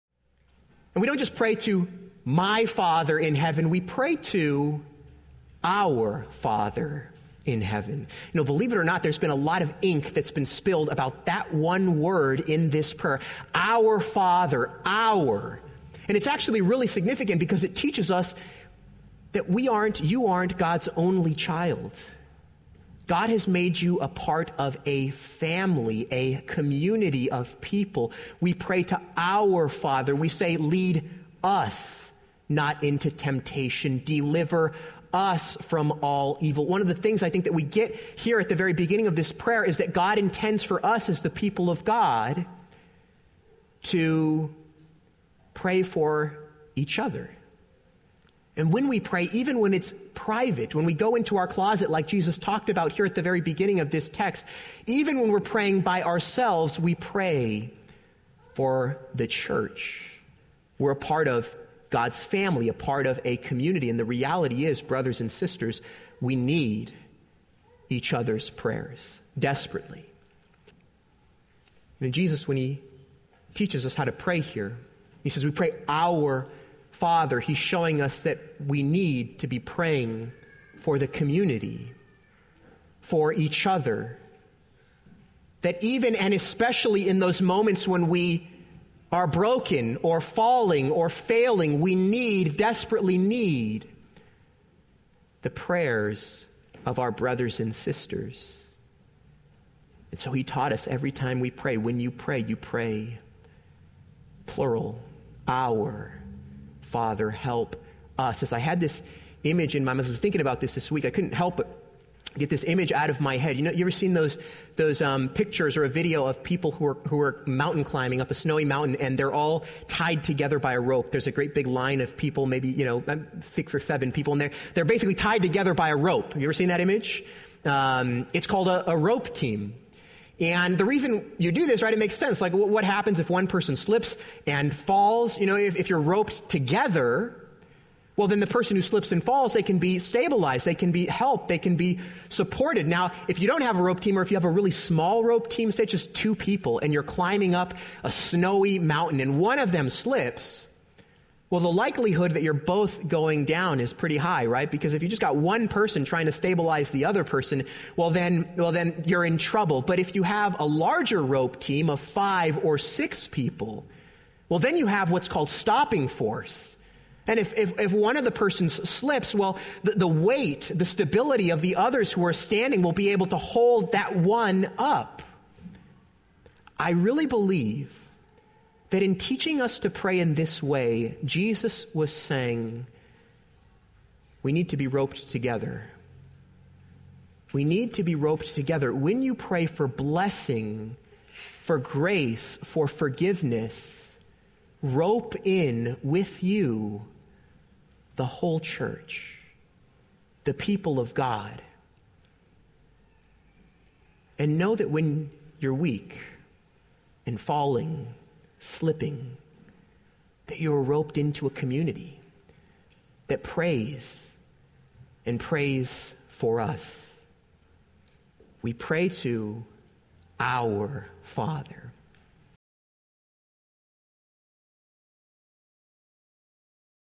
The Family of God
We don't just pray to 'my' Father in heaven, we pray to 'our' Father in heaven. When we are adopted by God, we are adopted into a family, into the community of God's people.